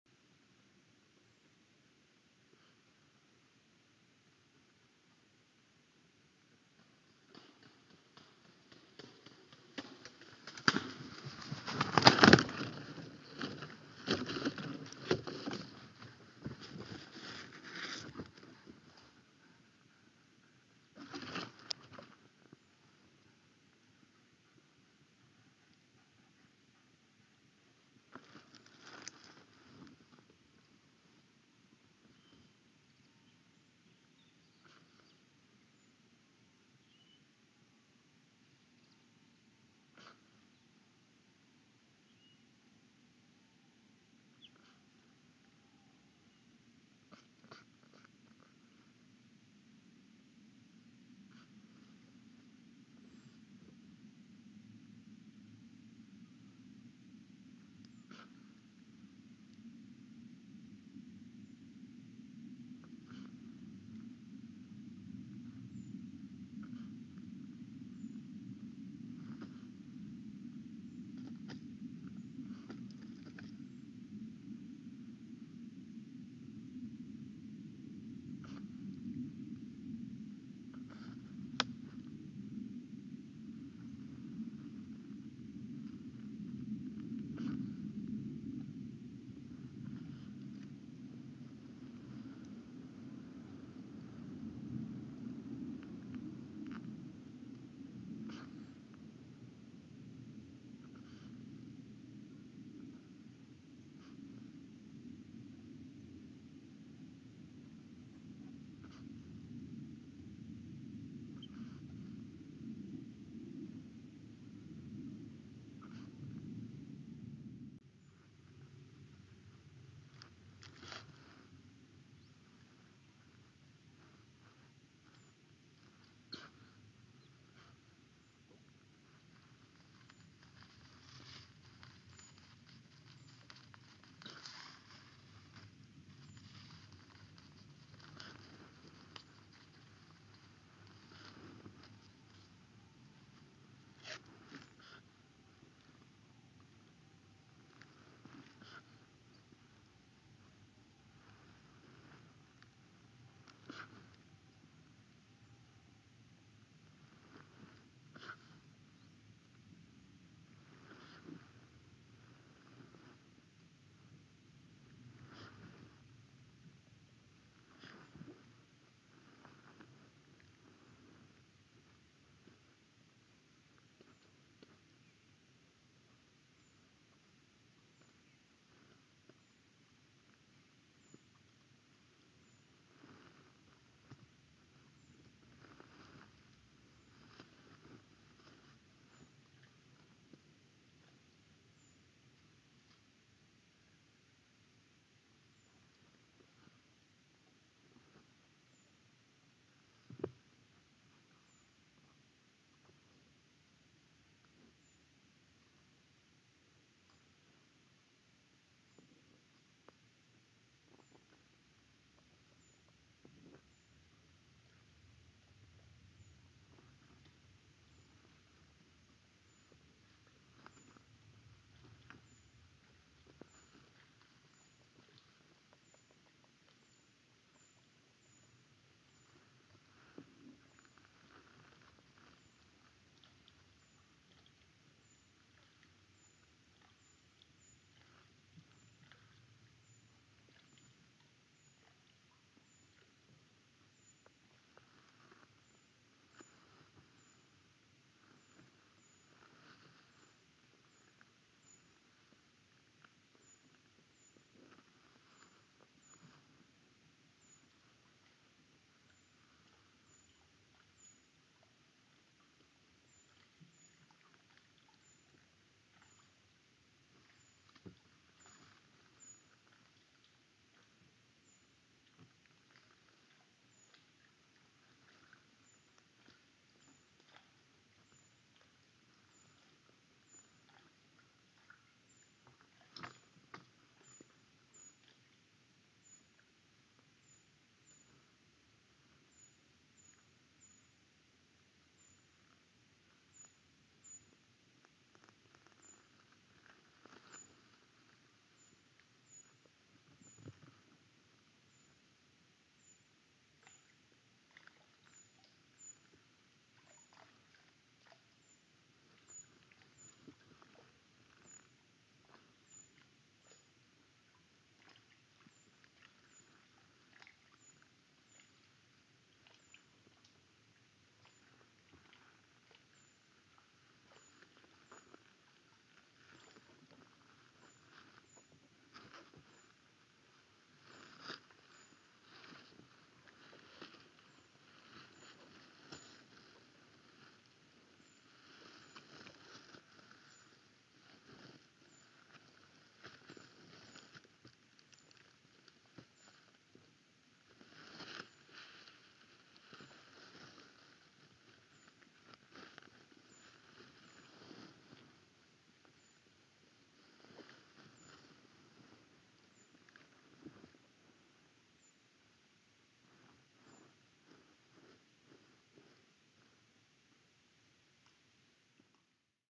Kotkaklubi. Merikotkas. The first egg has been laid. 🎊 🎉29.03.2023
Kotkaklubi
Merikotkas::Haliaeëtus albicilla